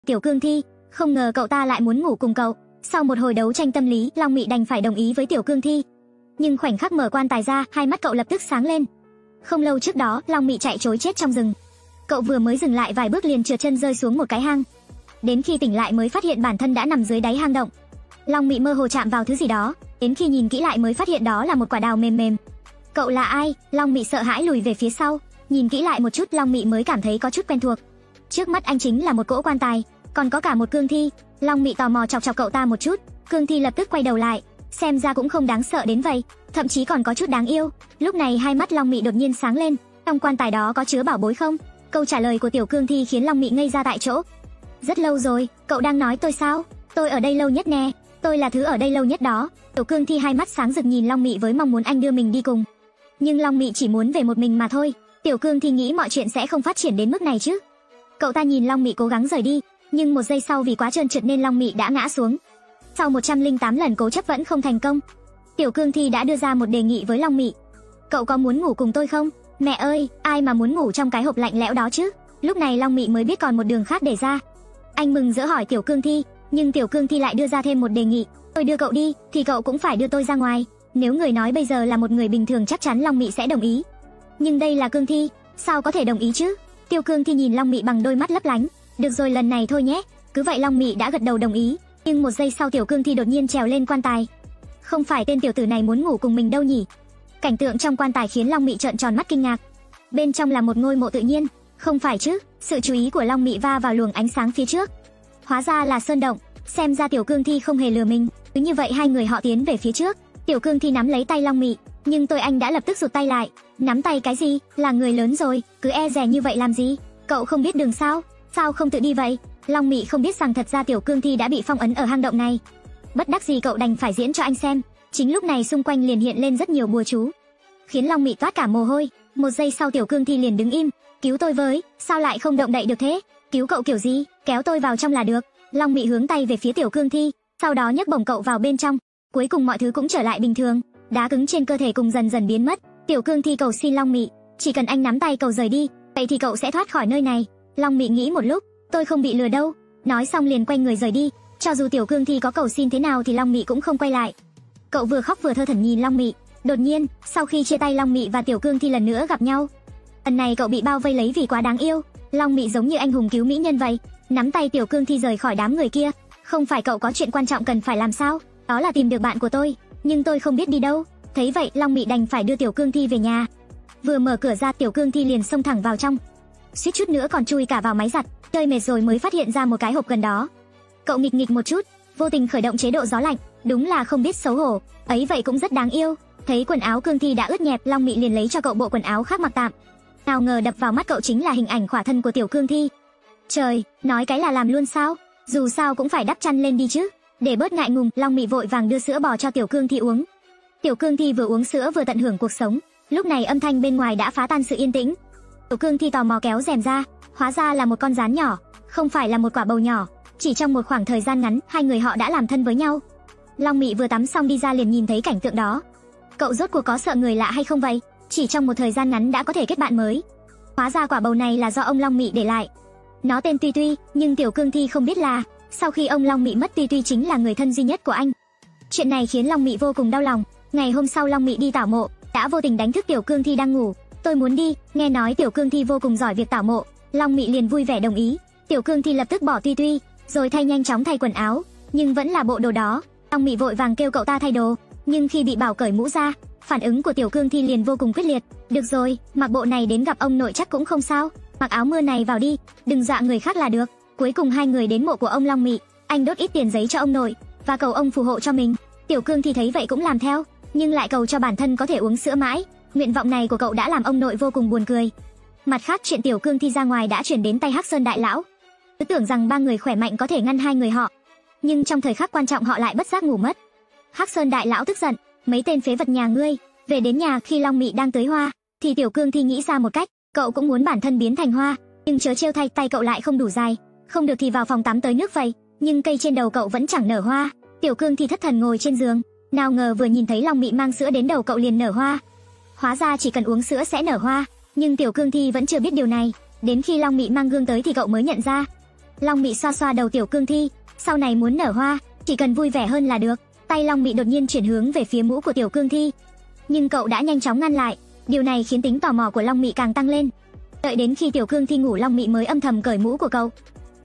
Cậu nhóc vô tình nhặt được một tiểu cương thiChạy 4 phương 8 hướng ko kịp còn bị rủ ngủ chung [Revi
Cậu nhóc vô tình nhặt được một tiểu cương thiChạy 4 phương 8 hướng ko kịp còn bị rủ ngủ chung [Review Truyện tranh]